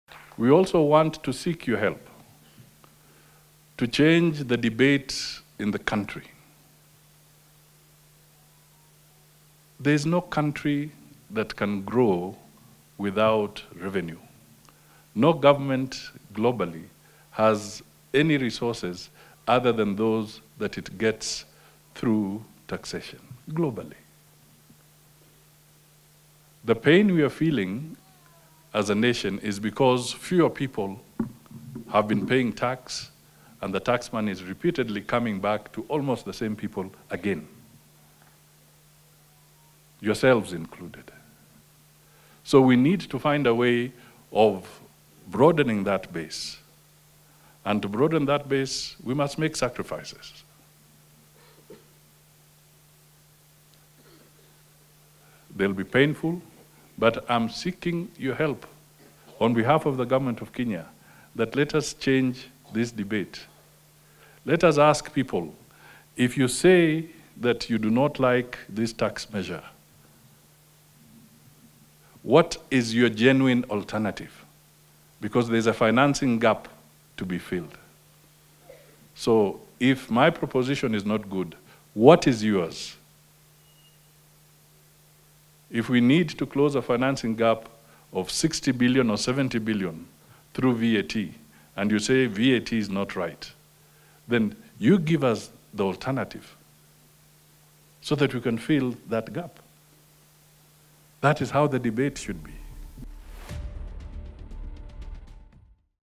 “We must make sacrifices,” Mudavadi tells Kenyans on the controversial Finance Bill 2023
Mudavadi asks Kenyans to make sacrifices as debate on the controversial Finance Bill 2023 rages on.